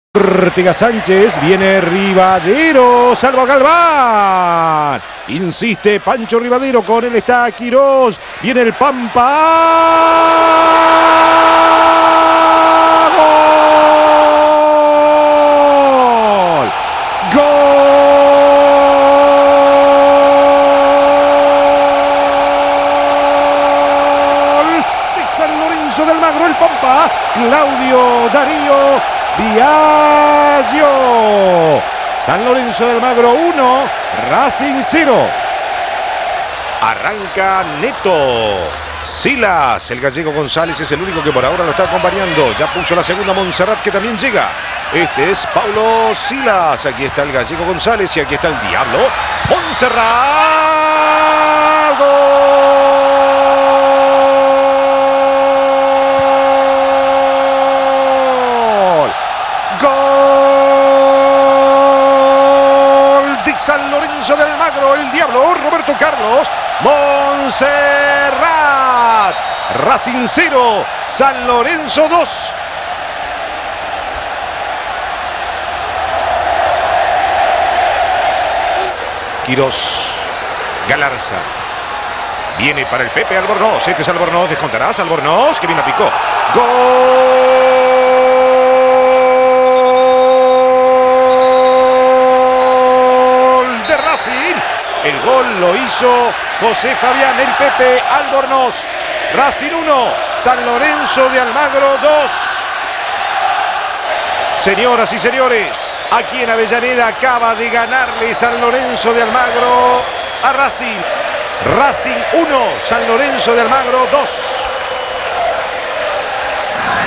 Racing 1 San Lorenzo 2 Clausura 1995 (Los goles ) (Relato Marcelo Araujo)
Los goles del partido con relato de Marcelo Araujo